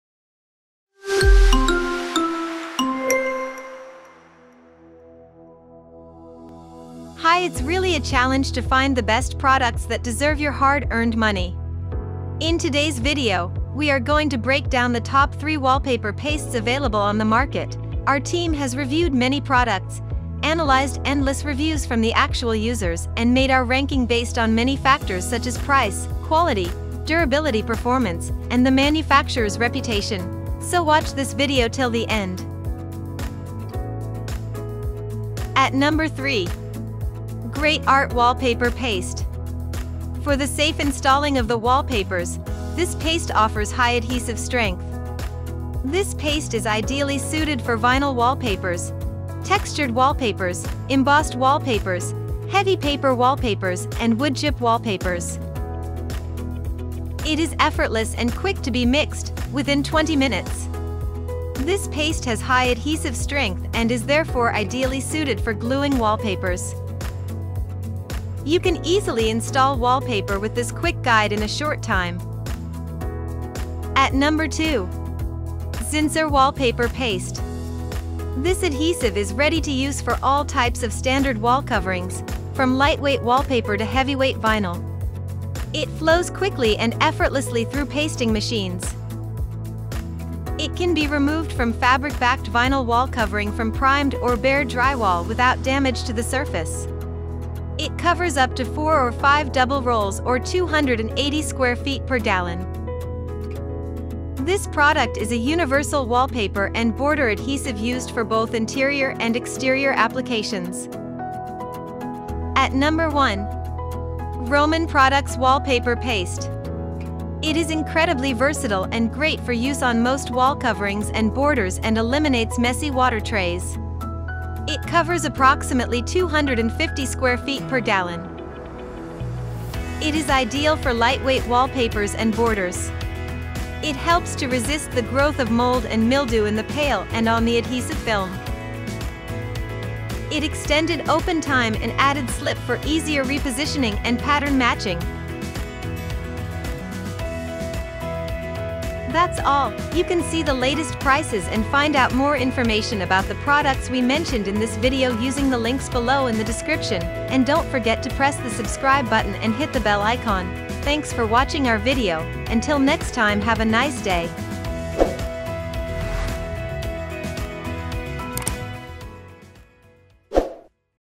Best Wallpaper Pastes
Follow the links given below to check the product prices:

 ROMAN Products - Wallpaper paste

Zinsser - wallpaper paste

GREAT ART - wallpaper paste

0:00​​  Best Wallpaper Pastes
0:35  GREAT ART - wallpaper paste
1:14  Zinsser - wallpaper paste
1:53  ROMAN Products - Wallpaper paste
2:28  Closing Remarks & Subscribe To Our ChannelFollow the links given below to check the product prices: